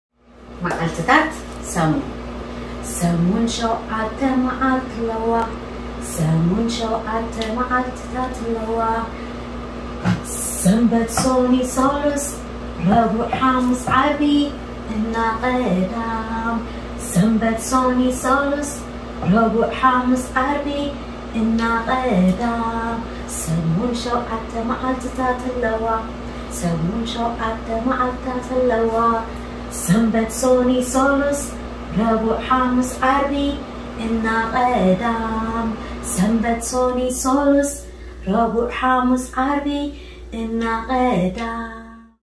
Tigrigna - Days of the Week
This rhyme is about the days of the week and their names.